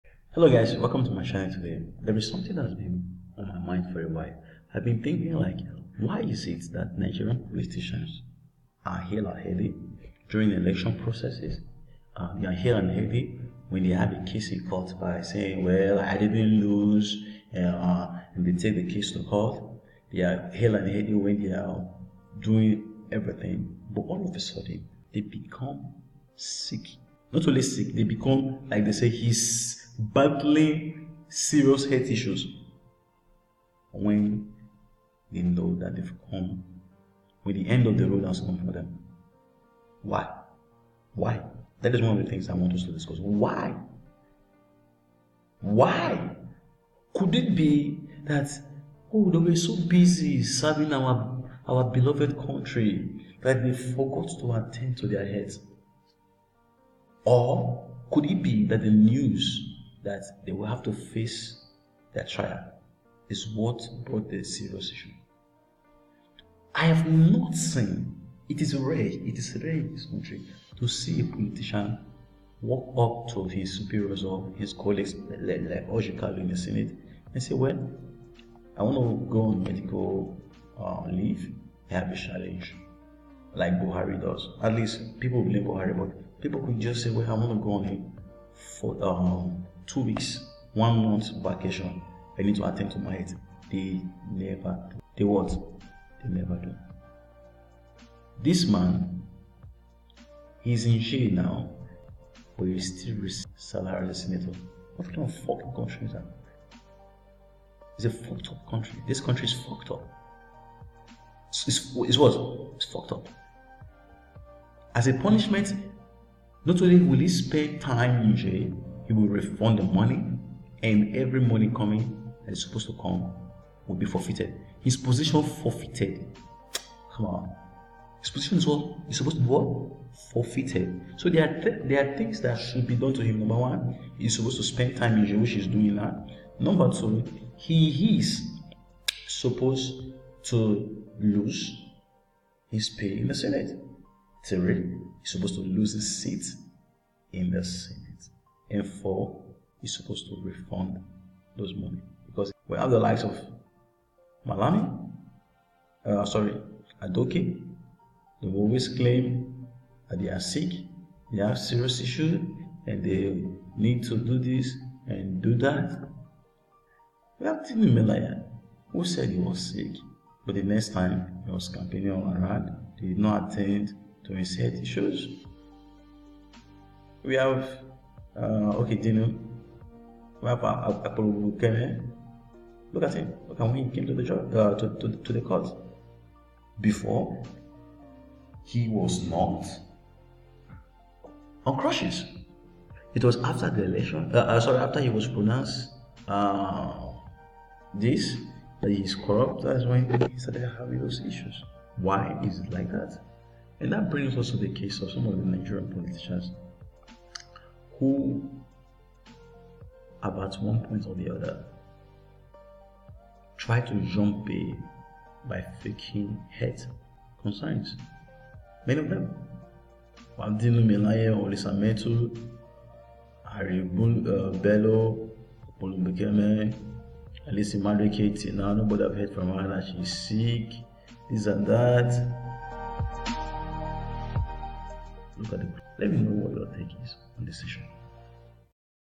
ORJI KALU DENIED BAIL|5 Nigerian politicians who spent Christmas in jail
Senator Orji Kalu To Spend Christmas In Prison As Court Dismisses Bail Application

Why are Nigeria politicians always sick when they are made to face their lots.
According to the Chairman, Presidential Advisory Committee Against Corruption (PACAC), Prof. Itse Sagay (SAN), some Nigerians feign sickness during a trial in an attempt to avoid the consequences of their alleged offence.

“I am worried by the attitudes of public officials who play the sick game whenever they are being tried. The only thing is from what I see on television, it is difficult for me to know whether it is genuine or fake.

“This is our attitude in this country. When you are engaging in impunity, you do it in full health but when payback time comes, then you are sick and you want to avoid the consequences of what you did with open eyes.

“This is what I’m always talking about. Let us do what is right and not now begin to look for excuses and promote sentiments when you are being visited by the law for the wrongdoings you committed. It is very unfortunate,”